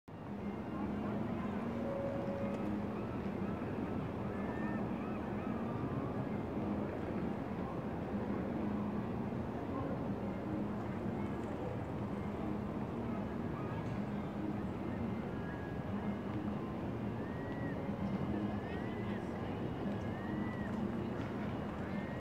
360 Video - Seattle Needle
A sunny day across from Key Arena...Seattle Needle in background.